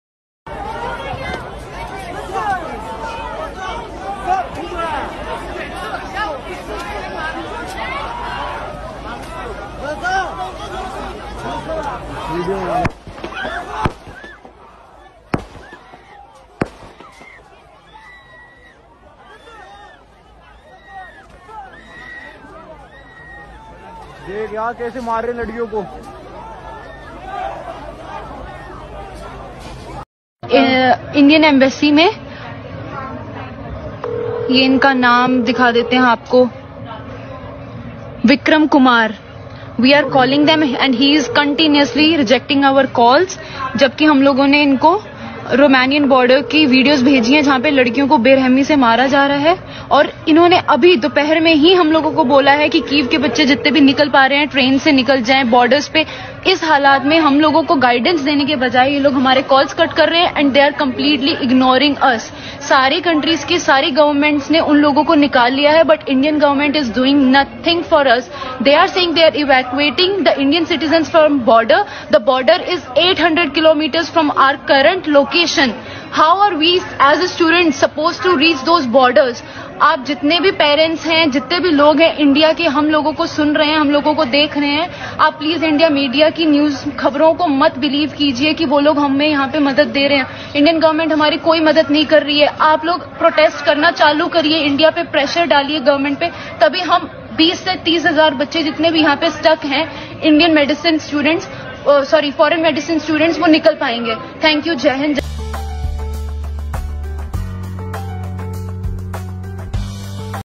RussiaUkraineConflict | युक्रेन-पोलंड सीमेवर भारतीय विद्यार्थ्यांना मारहाण; पाहा व्हिडीओ....
RussiaUkraineConflict | युक्रेन-पोलंड सीमेवर भारतीय विद्यार्थ्यांना मारहाण, दुतावासानेच बोलावलं पण अधिकारी गैरहजर,फोनही उचलत नसल्याचा विद्यार्थ्यांचा आरोप